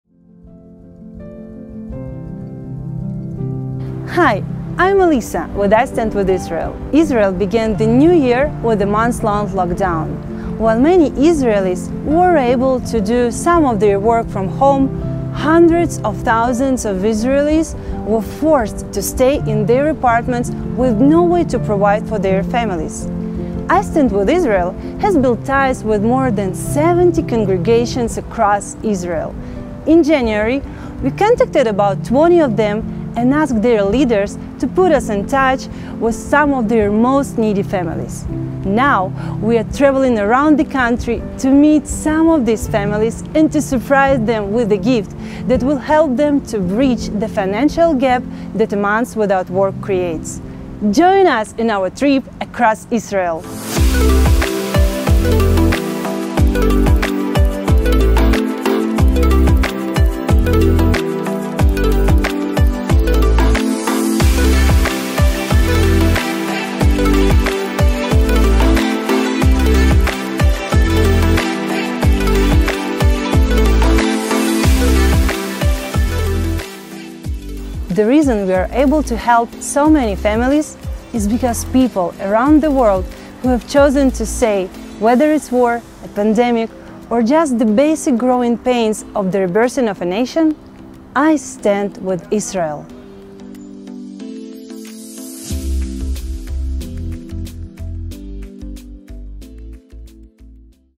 2021 Emergency Funding for Israeli Families
Your support made it possible for I Stand with Israel to help these families in need during the lockdowns in Israel. Every month, we are tuned in to the needs surrounding us here in the Land—for both Jews and Arabs in the Body of Messiah. We are passionate about making believers STRONG in Israel. Those lives would not have been changed without the magnificent partnership we have with you.  Your giving... your praying... your supporting... all of that matters—to God, to us, and to all of these families.